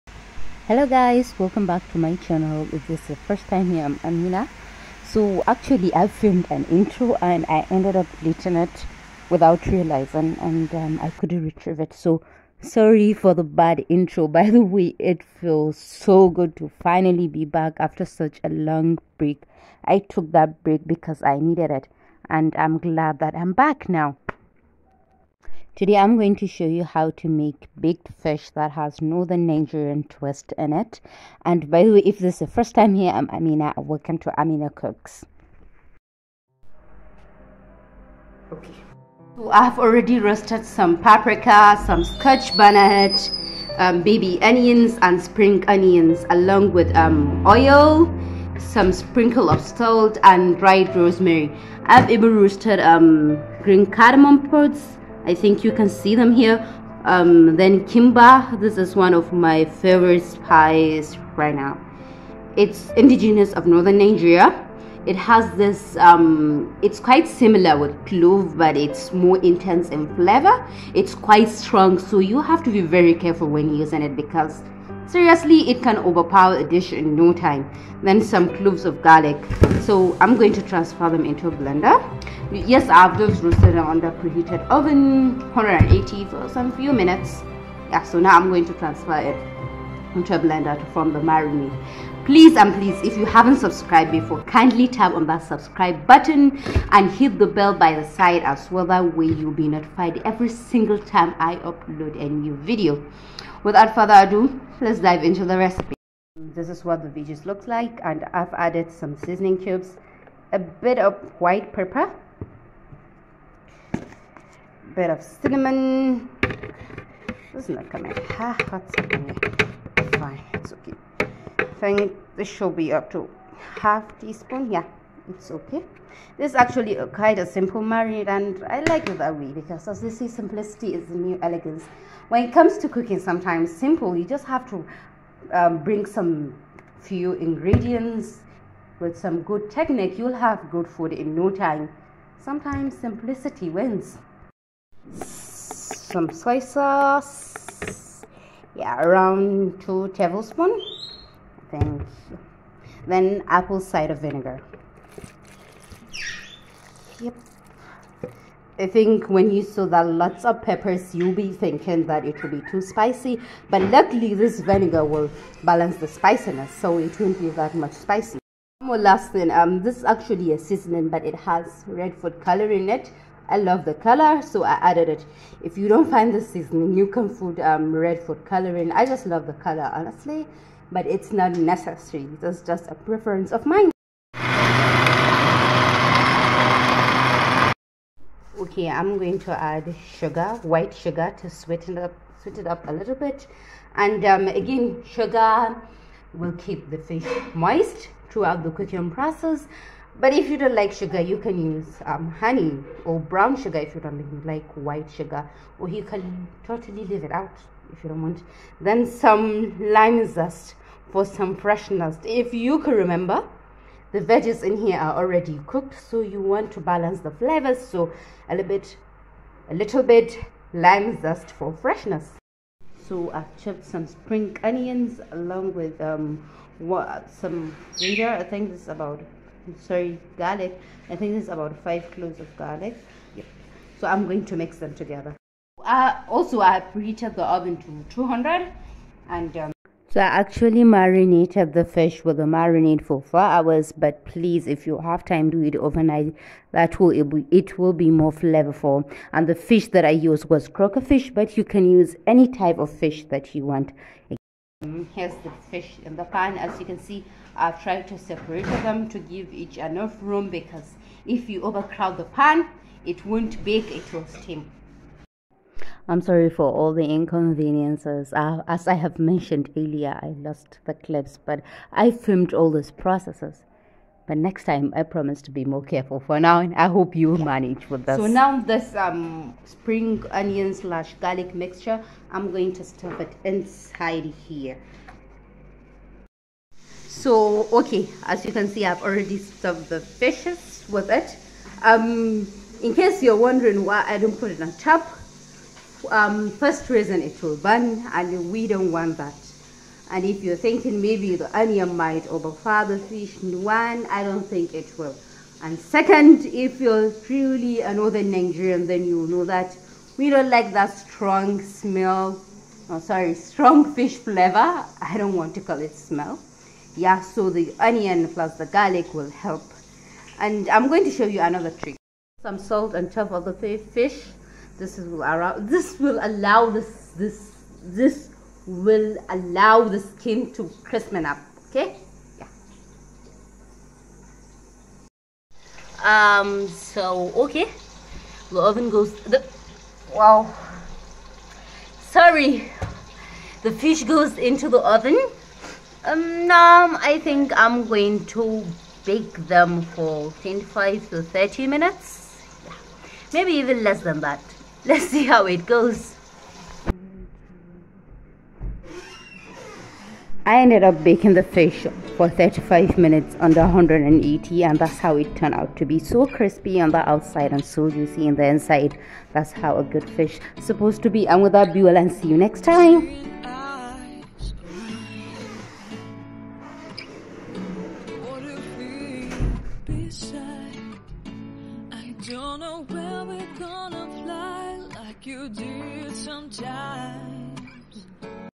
Best Croaker Fish Recipe!| Heartwarming Dinner in 35 minutes !
Hello guys, how have you all been?
I hope you are all doing well and in the best of health.

In today's episode, im going to show how to make the best Baked croaker fish you have ever eaten. It's so juicy and flavorful in the inside and crispy at the outside, i hope you will love it.

Full measurements of everything is already on the blog, you can tap the link below for it

I'm here every week to bring new exciting flavours into your kitchen.